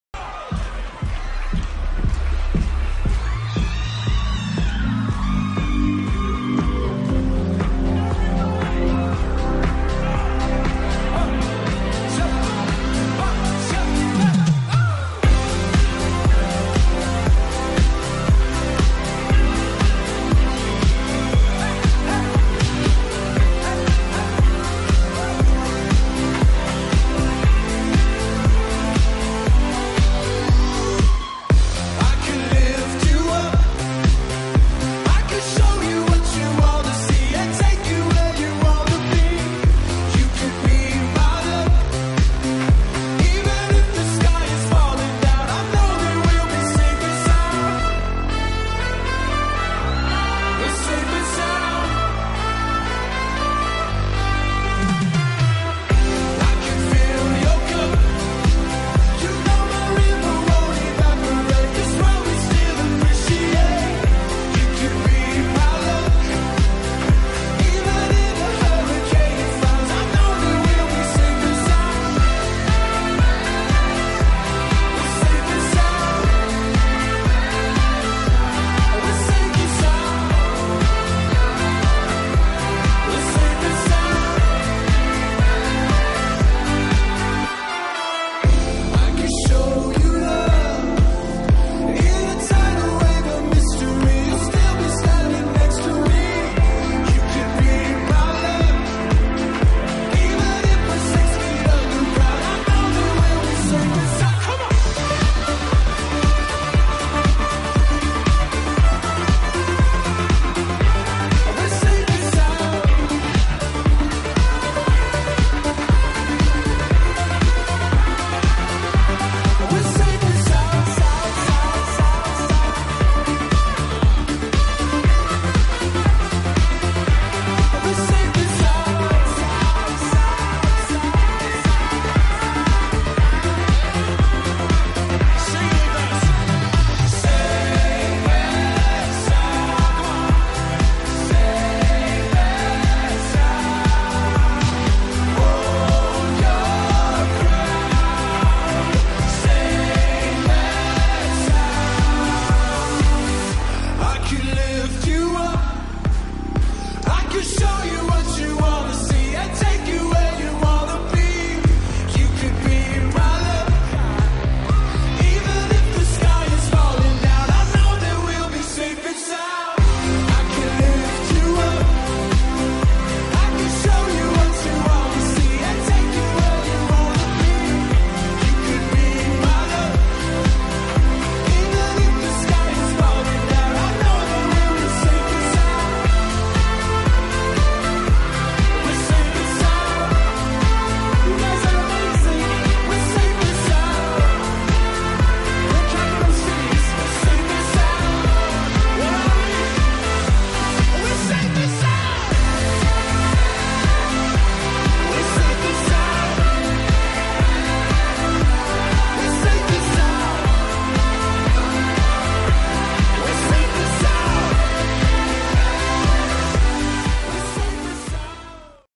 CTBUH 2015 NYC!
Photos from the Council on Tall Buildings and Urban Habitat's 2015 International Conference in  New York.

Written and performed by Capital Cities
Label: Lazy Hooks / Capitol